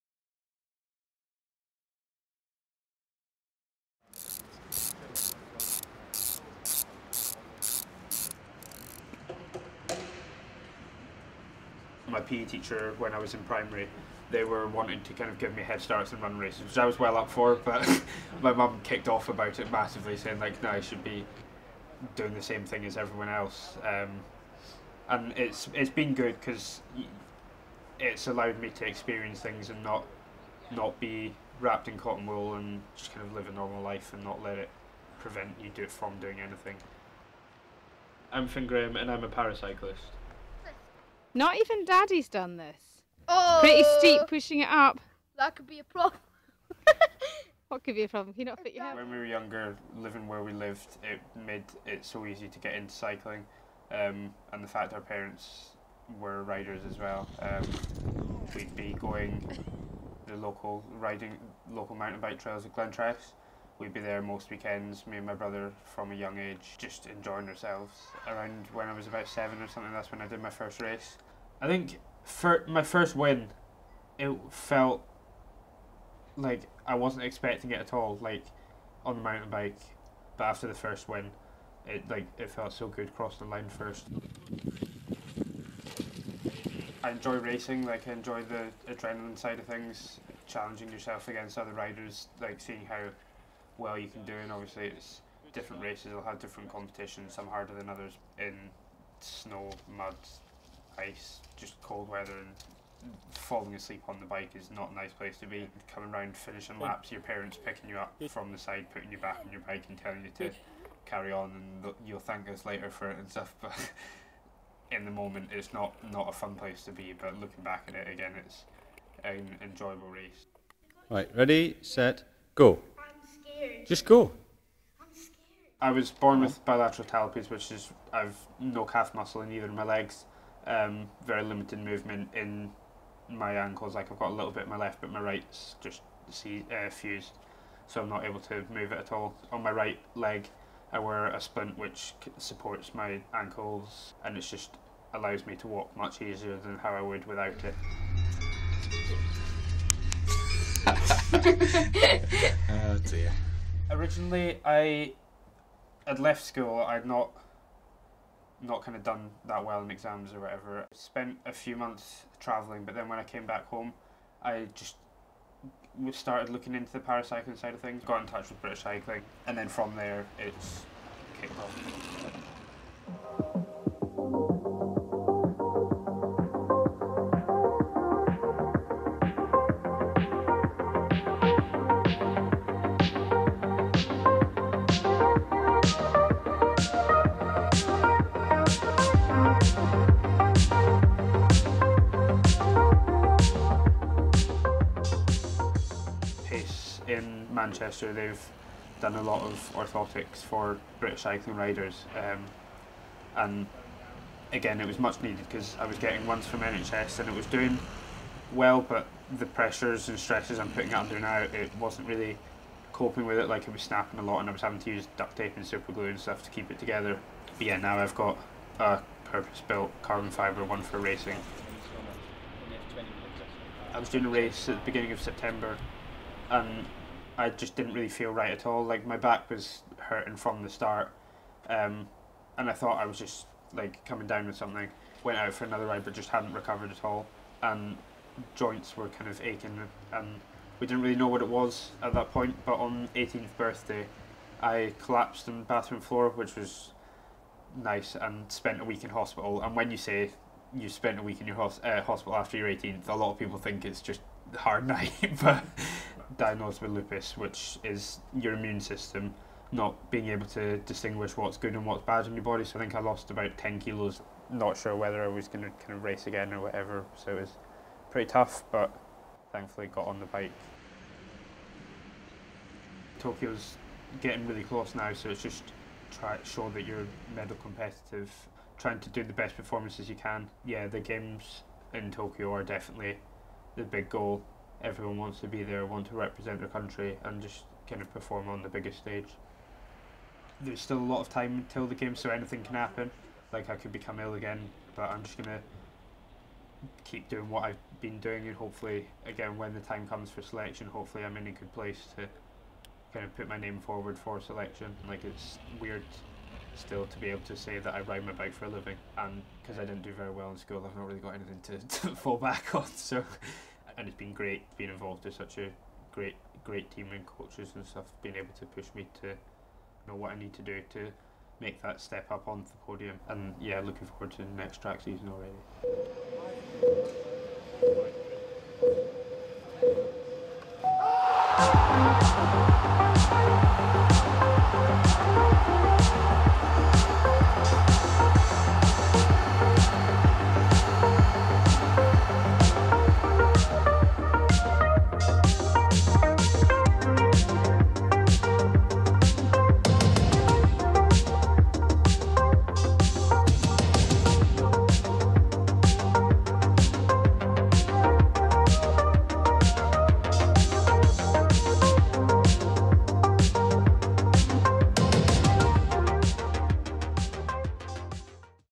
SPLINT: Para-cycling Documentary
Fin Graham from Team GB tells his story of perseverance in the face of adversity, overcoming disability and illness in order to compete for a place on the cycling track at the 2020 Tokyo Paralympic Games